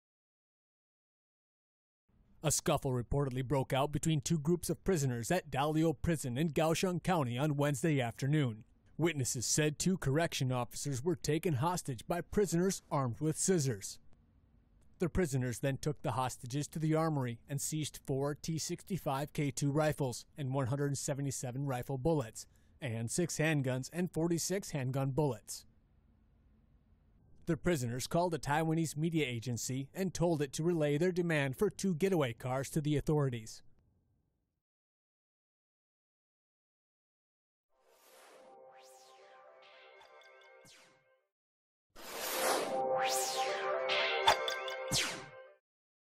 Taiwan prison inmates take warden hostage in escape attempt
Originally published on 11 February, 2015

Six inmates at a prison in southern Taiwan have taken the warden and head prison guard hostage in an escape attempt.According to the official Central News Agency, a scuffle reportedly broke out between two groups of prisoners at Daliao Prison in Kaohsiung county on Wednesday afternoon. Witnesses said two corrections officers were taken hostage by prisoners armed with scissors.The prisoners then took the hostages to the armoury and seized four T65K2 rifles and 177 rifle bullets, and six handguns and 46 handgun bullets.The prisoners called a Taiwanese media agency and told it to relay their demand for two getaway cars to the authorities.Among the six inmates, who are mostly serving sentences for drug possession and robbery, is Cheng Li-teh, a leader of a local triad known as the Bamboo Union.The inmates originally captured head guard Wang Shih-tsang and Deputy Warden Lai Chen-jung. Warden Chen Shih-chih volunteered to take Lai’s place, and Lai was freed. Negotiations are still underway.